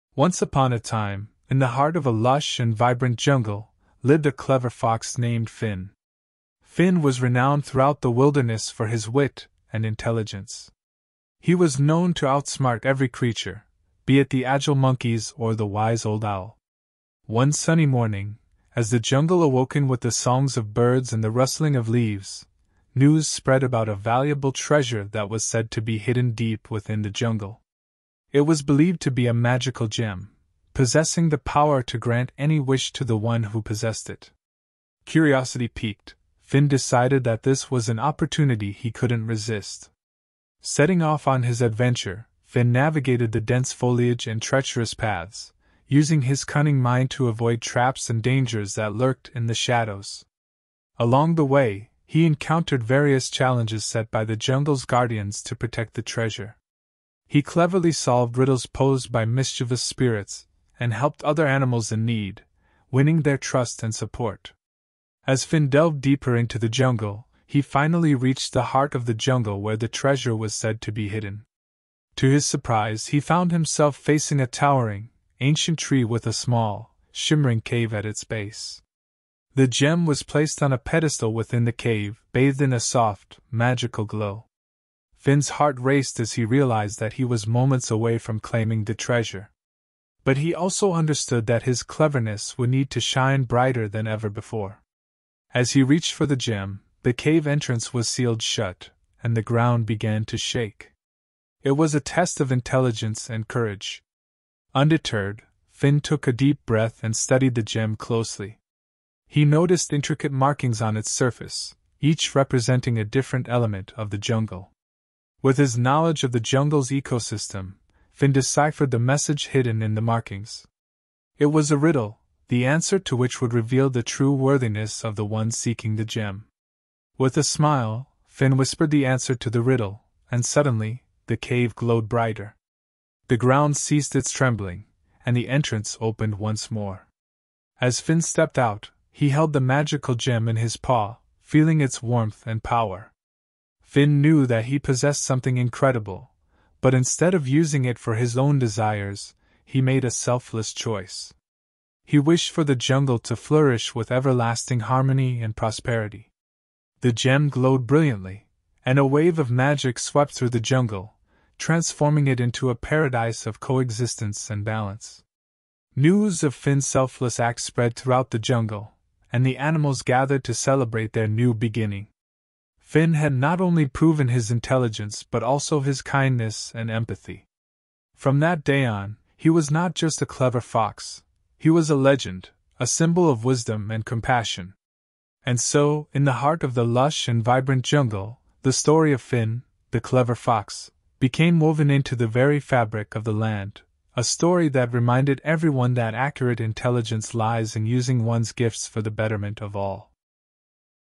Learn English Through Story | Cleaver Fox Magical Discover | Improve English Reading & Listening
Welcome to Speak in Stories, where learning English comes to life through captivating narratives. Join us on a journey of language and imagination with "Clever Fox's Magical Discovery." Our carefully crafted story not only entertains but also enhances your English reading and listening skills. Immerse yourself in this enchanting tale and watch your language proficiency grow.